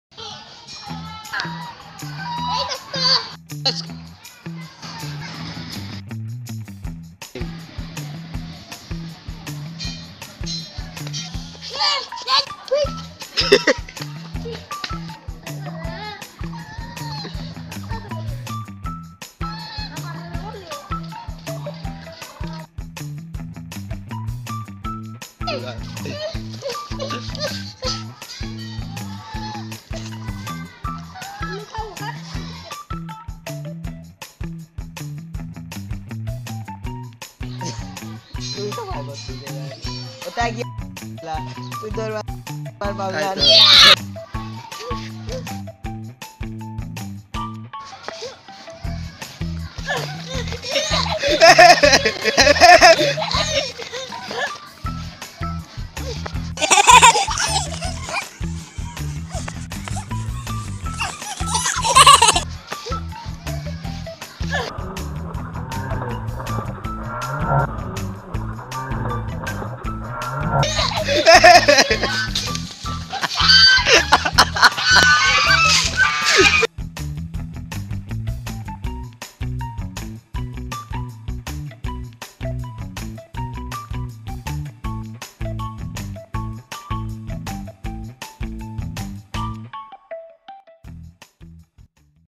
Very Funny Goose Chasing Kids compilation 😂 | Funniest Animal Video 2020 | New funny video
Very Funny Goose Chasing Kids compilation, Funniest Animal Video 2020.

• I am not Owner of these Music in this video.